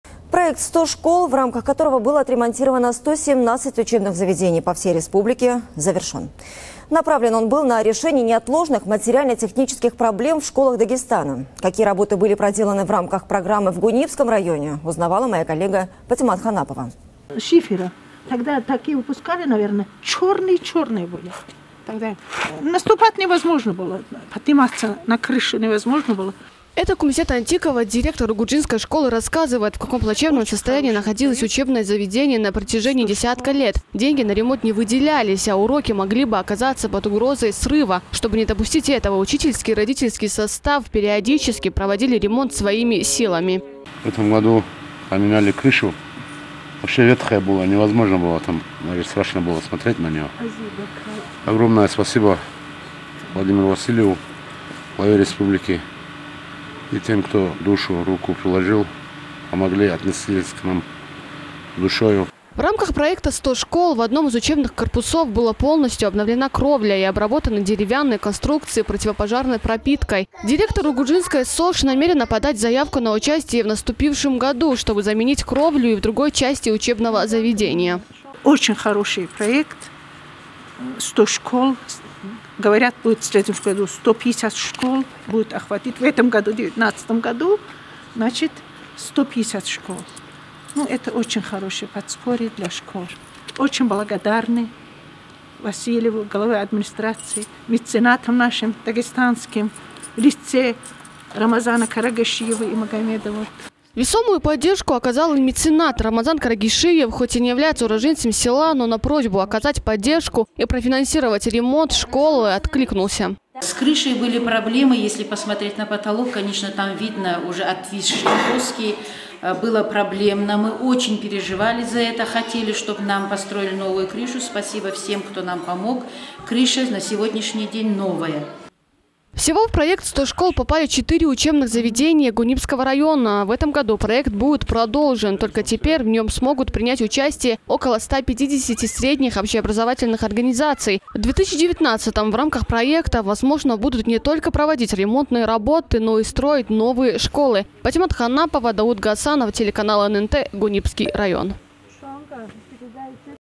Проект «100 школ» в Гунибском районе реализован
Проект 100 школ, в рамках которого было отремонтировано 117 учебных заведений по всей республике завершен.  Направлен он был на решение неотложных материально- технических проблем в школах Дагестана. Какие работы были проделаны в рамках программы в Гунибском районе смотрите в сюжете.